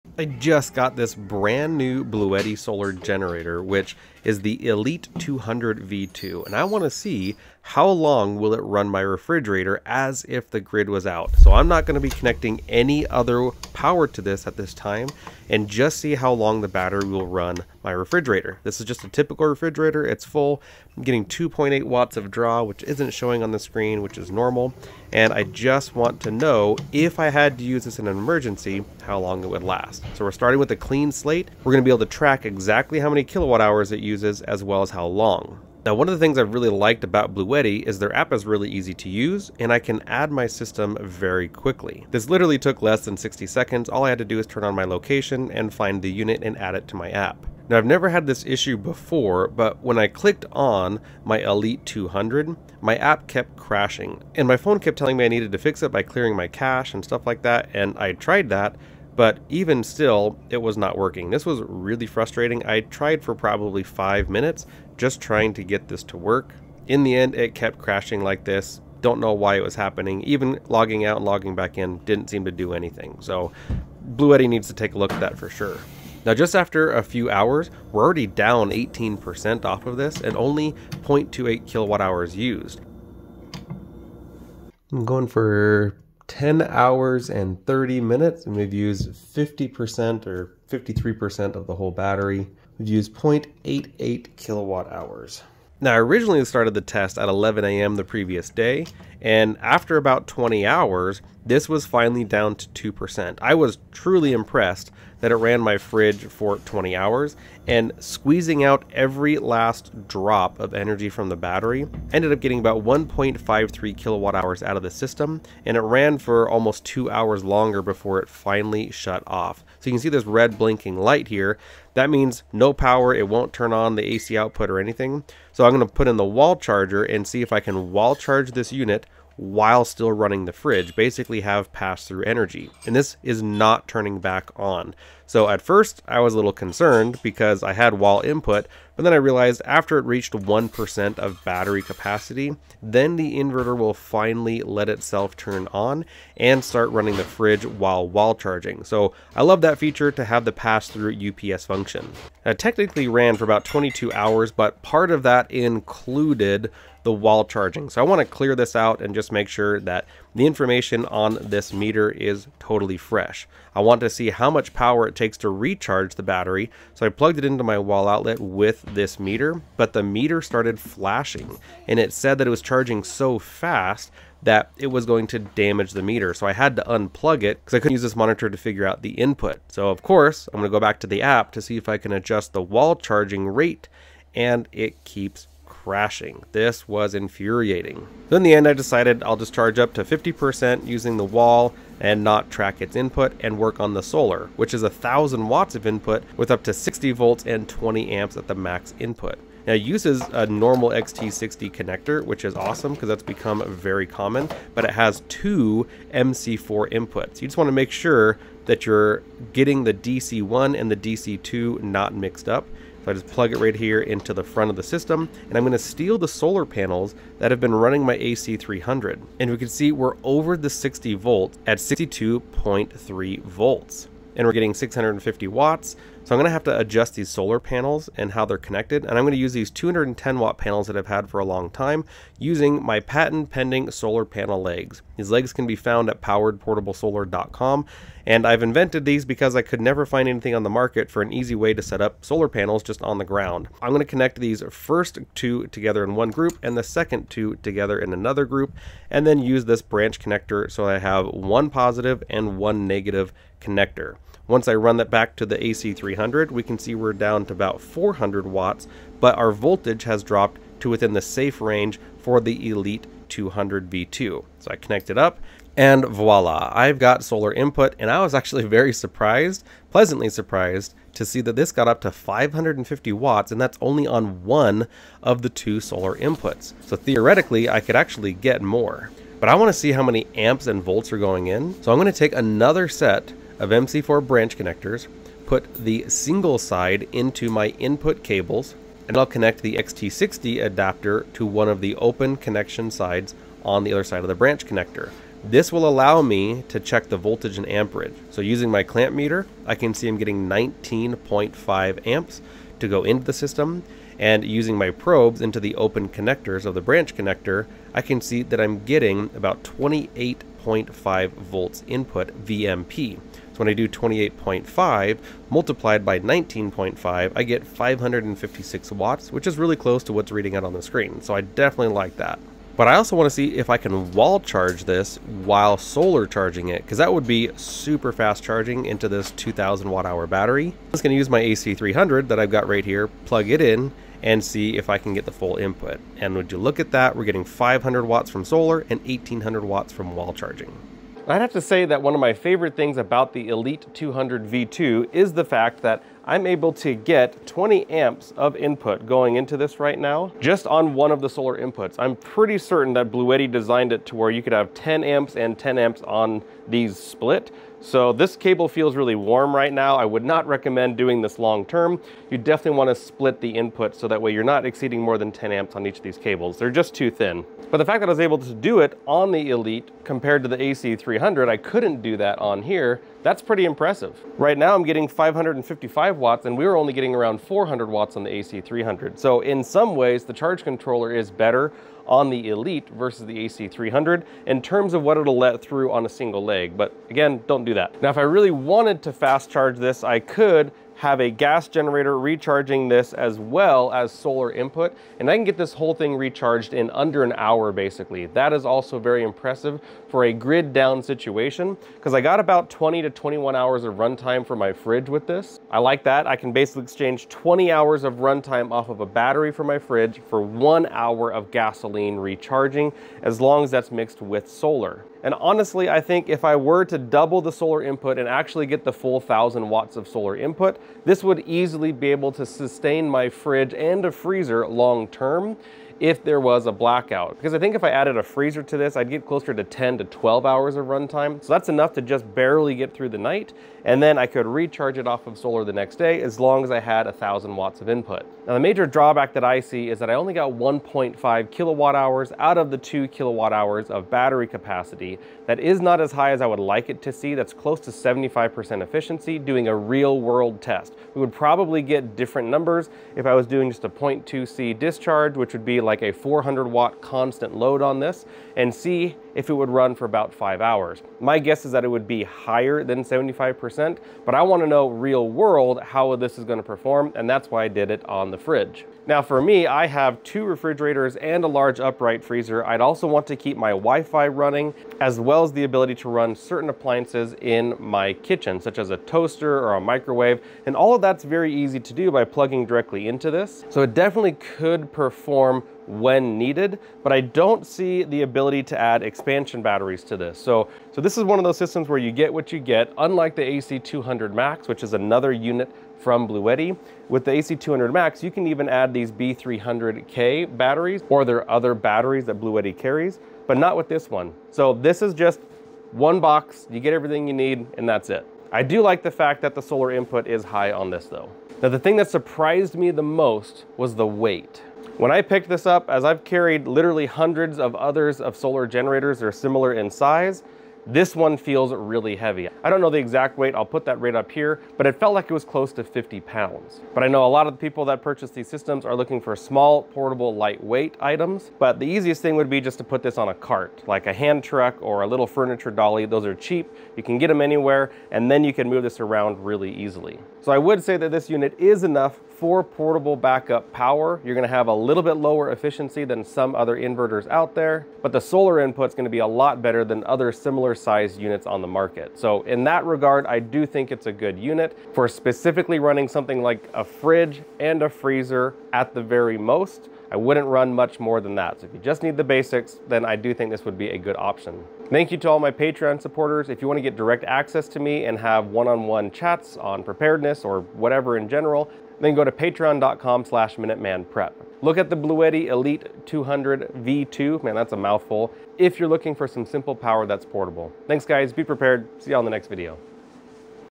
Bluetti Elite 200 V2 Portable Solar Generator Review
Check out the brand new Bluetti Elite 200 V2 solar power station. This unit has a densely powerful 2 kWh battery and will even support up to 1 kW of solar input. That's WAY more than the vast majority of the competitors with units of this size.

BLUETTI Black Friday Deals on Amazon